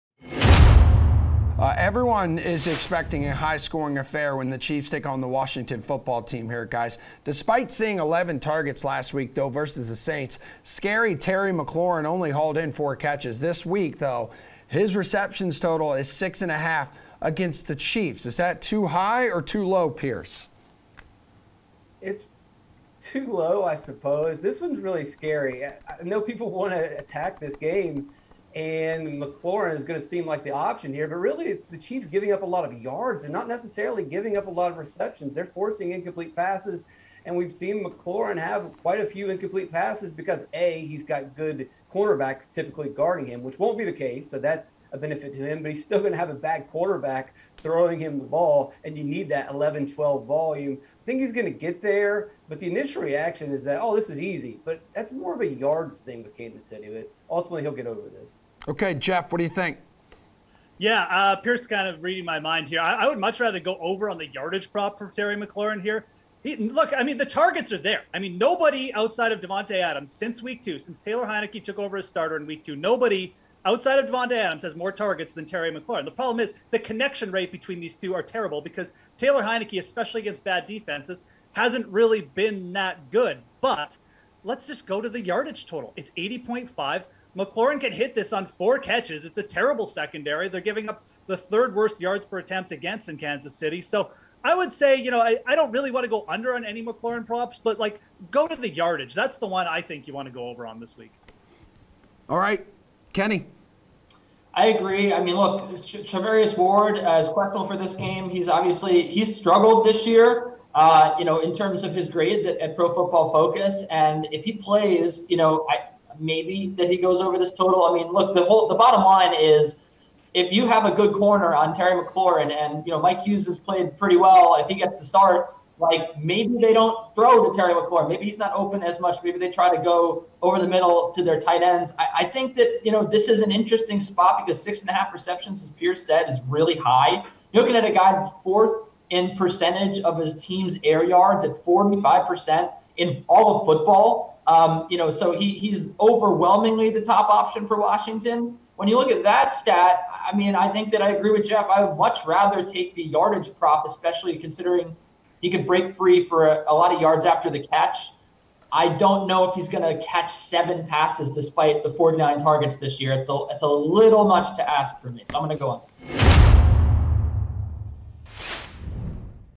How many receptions will Terry Mclaurin have against Kansas City?
DraftKings Contributor Pearce Detrich, Geoff Ulrich, and Kenny Ducey join the Sweat to discuss if they think Terry Mclaurin will go over his total receptions prop available on the DraftKings Sportsbook for Week 6 of the NFL Season